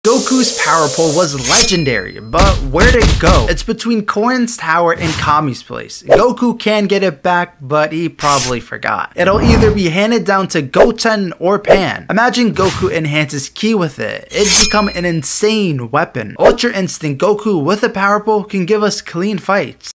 What Happened to the POWER POLE?! | Dragon Ball Super
Goku loved using the power pole, but randomly stopped. It was a great weapon from Dragon Ball and Dragon Ball Z! Maybe we see it in the Dragon Ball Super manga.